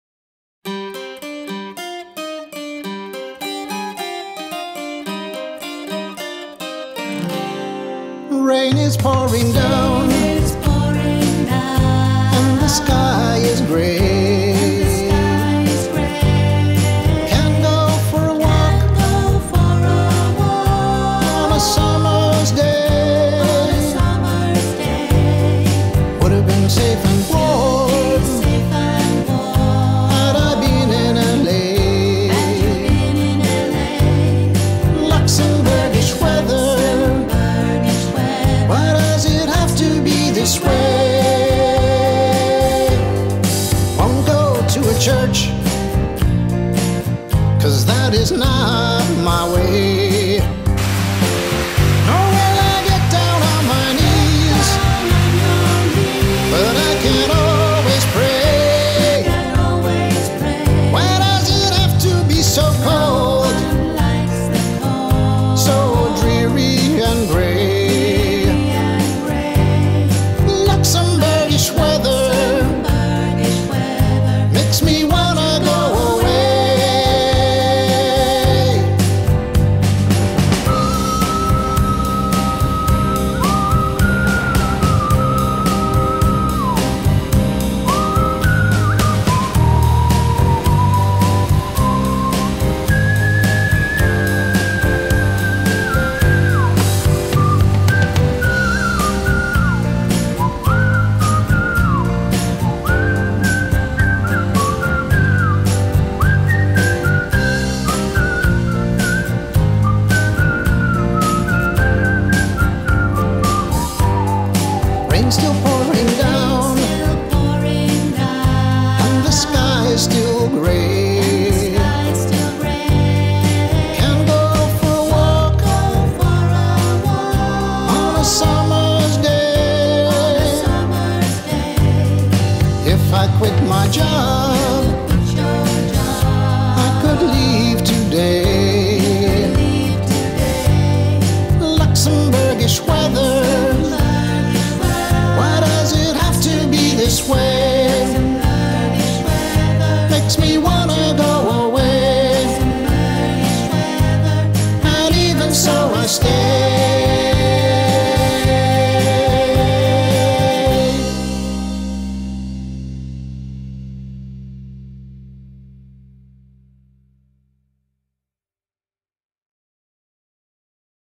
Luxembourgish Weather
Luxembourg is a great place to live, for many different reasons. But there is a downside ...

BTW if you like this video, there are more like it on the Peter J. Piper Parody channel Or you can use the playlist